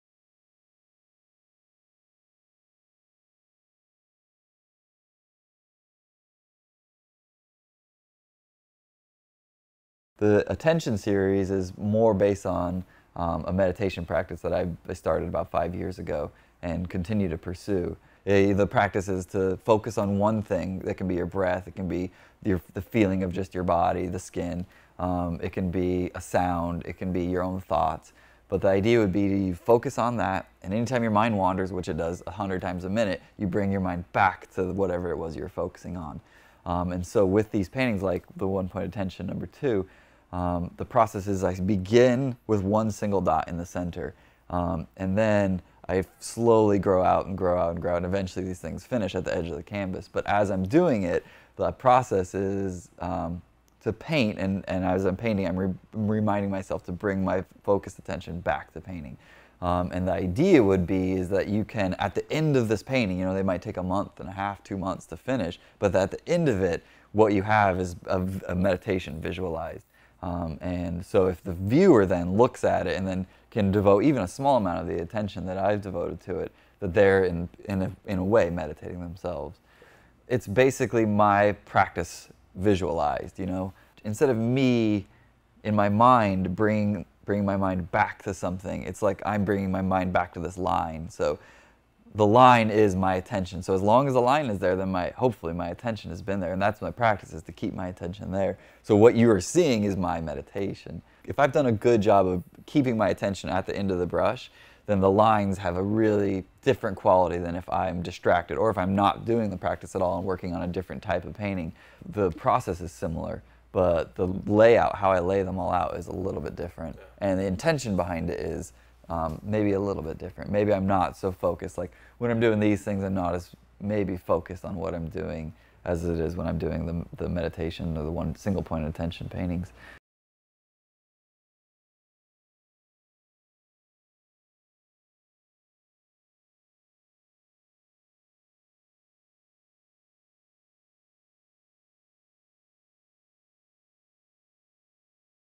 Kelsey Brookes– Intention as Meditation
Kelsey Brookes talks about the Intention series and how it represents and facilitates his meditation practice.